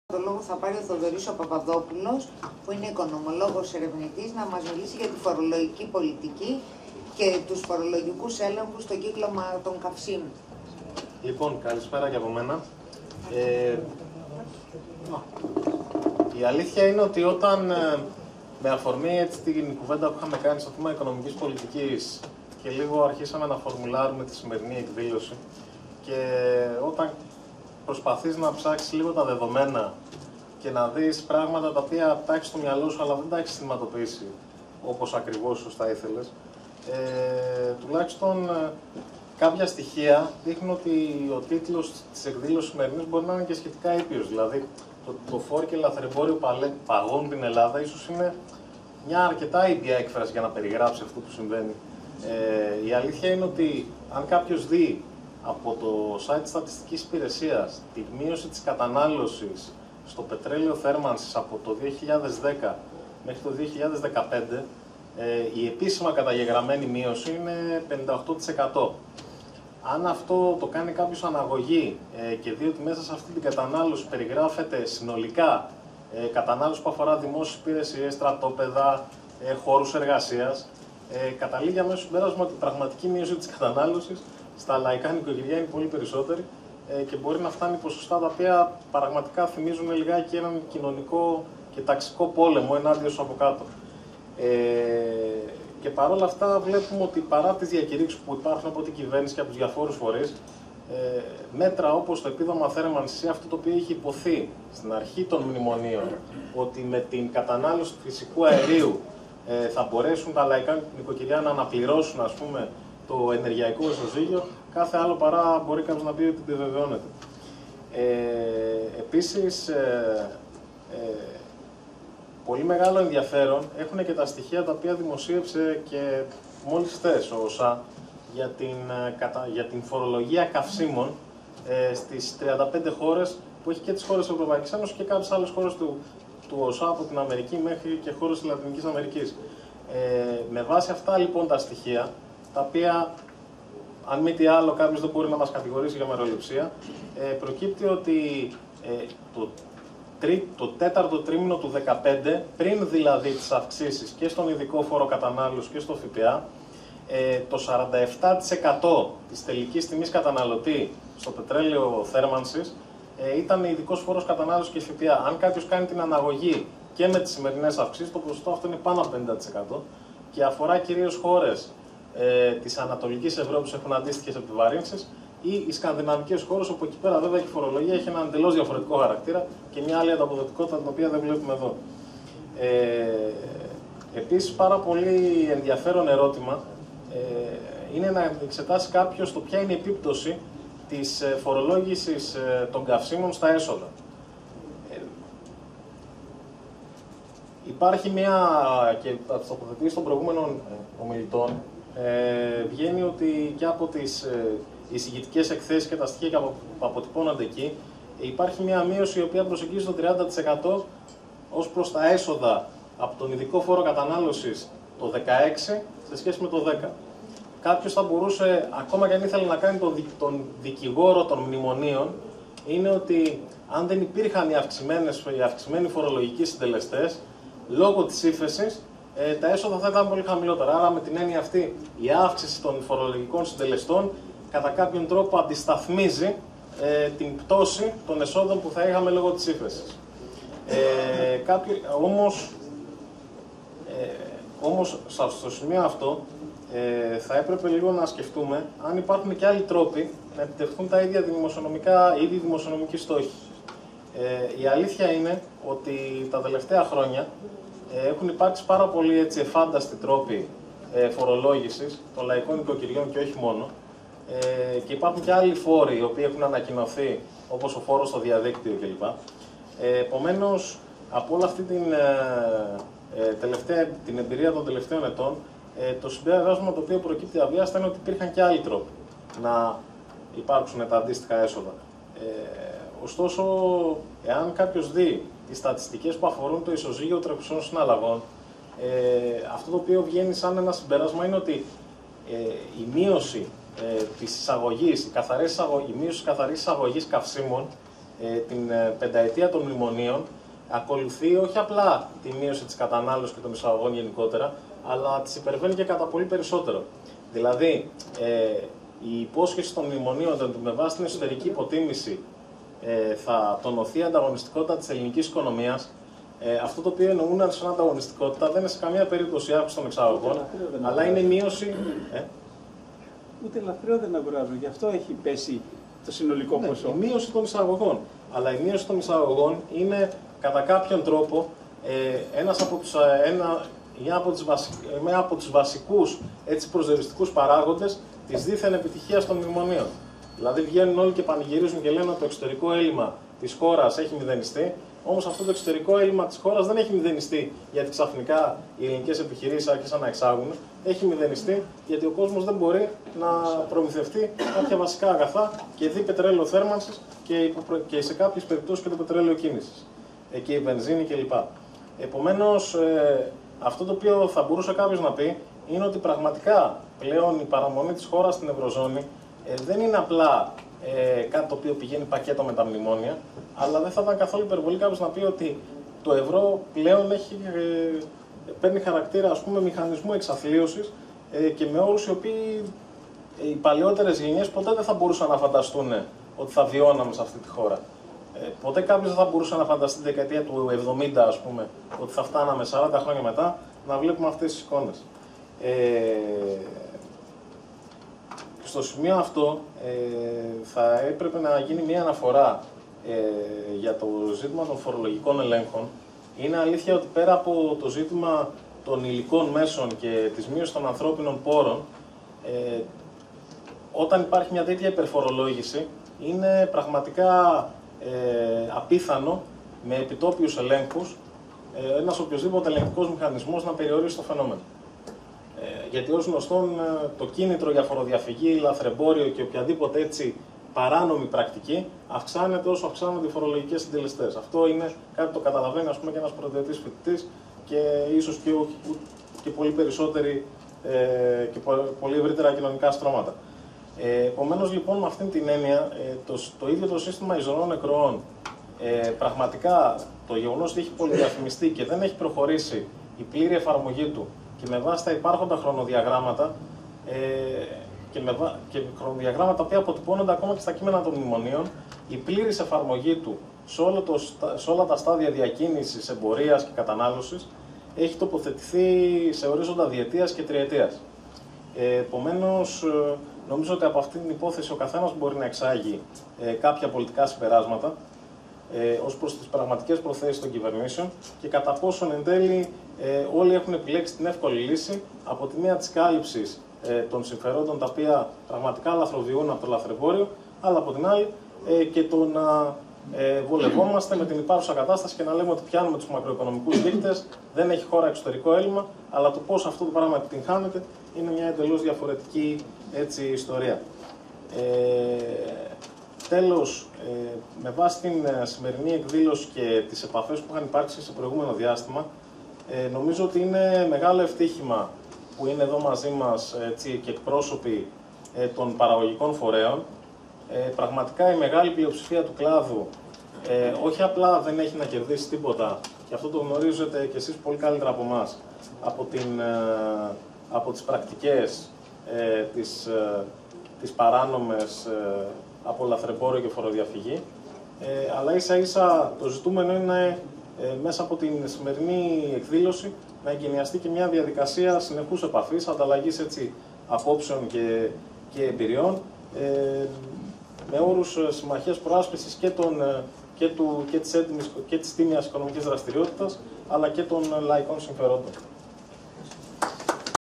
Θ. ΠΑΠΑΔΟΠΟΥΛΟΣ- ΗΜΕΡΙΔΑ ΛΑΕ - ΚΑΥΣΙΜΑ, ΛΑΘΡΕΜΠΟΡΙΟ 1.12.2016
«Φορολογική πολιτική και φορολογικοί έλεγχοι στο κύκλωμα καυσίμων»
Του Θοδωρή Παπαδόπουλου*

ΗΜΕΡΙΔΑ

«Λαθρεμπόριο Καυσίμων - Φορολογική πολιτική - Θέρμανση λαϊκών νοικοκυριών»

Ημερίδα: της «Λαϊκής Ενότητας»:

Πέμπτη 1 Δεκέμβρη 2016, 6.30μμ Αίθουσα ΕΣΗΕΑ- Αθήνα